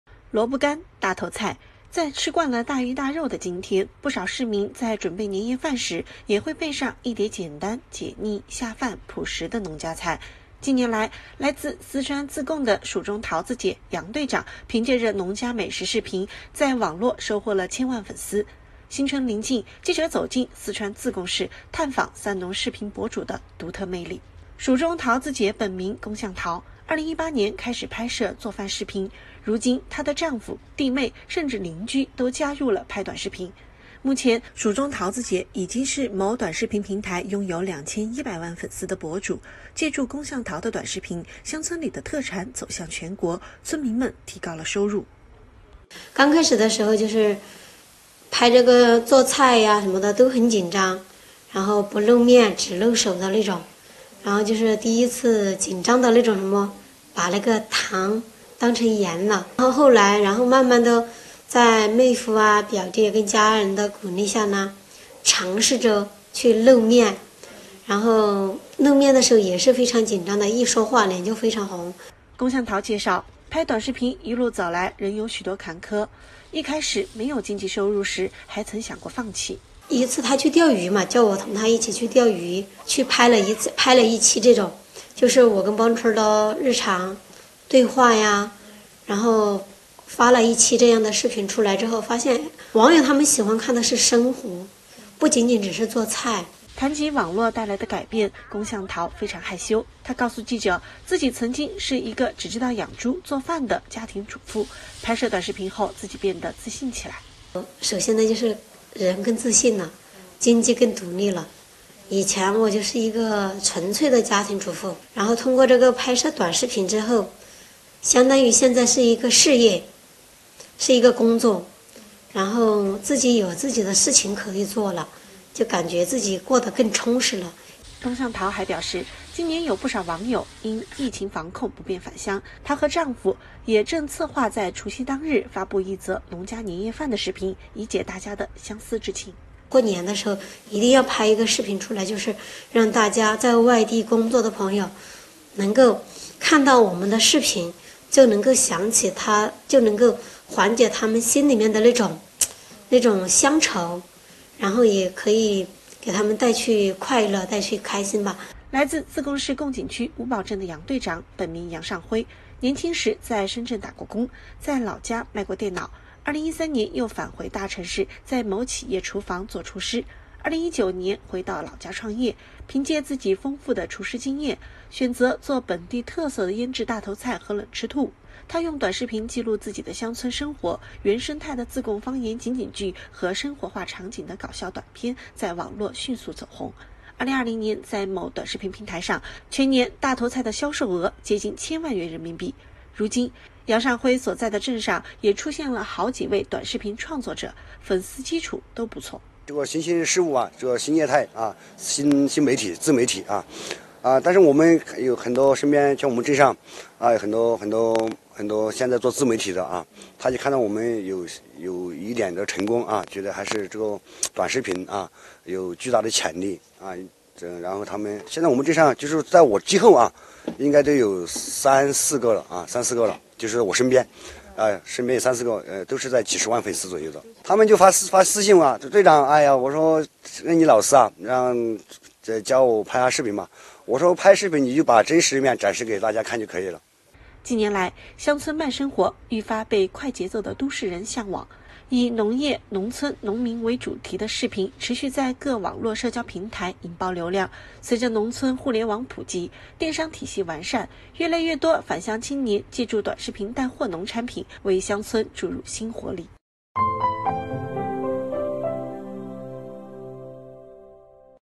千万视频博主预拍农家年夜饭 解众人思乡情
萝卜干、大头菜……在吃惯了大鱼大肉的今天，不少市民在准备年夜饭时，也会备上一碟简单、解腻、下饭、朴实的农家菜。近年来，来自四川自贡的“蜀中桃子姐”、“杨队长”凭借着农家美食视频，在网络收获了千万粉丝。新春临近，记者走进四川自贡市，探访三农视频博主的独特魅力。